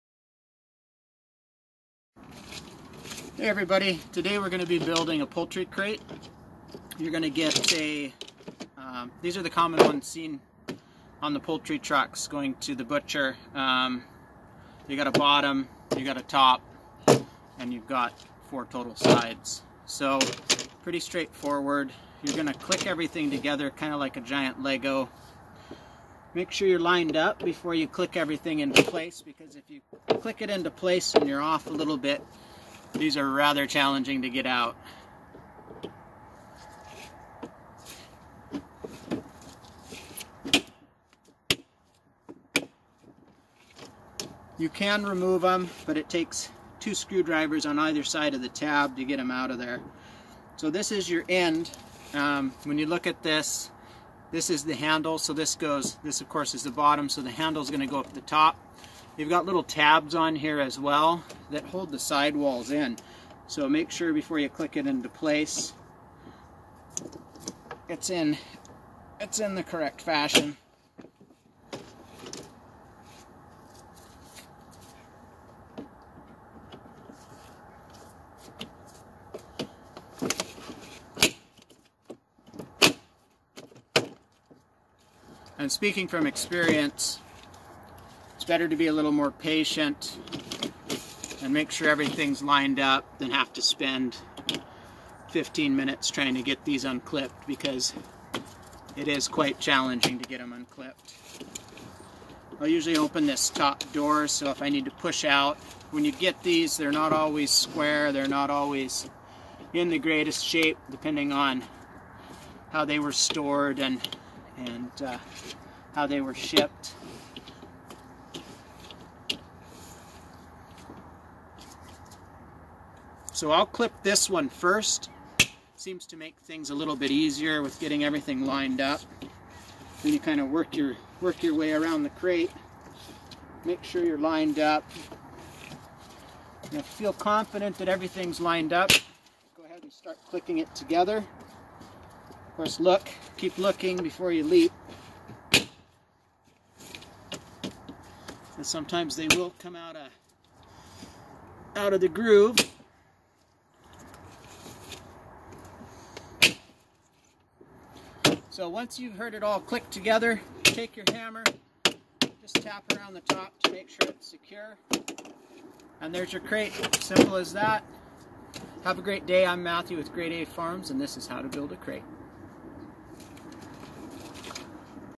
Crate Building 101: How to Assemble a Poultry Transport or Carrier Crate for Chickens
How to build a poultry transport crate.  This poultry crate is used for transporting birds safely.  It is a simple installation.  In this video, Matthew shares some tips on how to make assembly easier.  He also discusses what to do or how to disassemble if you make a mistake in locking parts together.    This is a really useful video on Crate Building 101.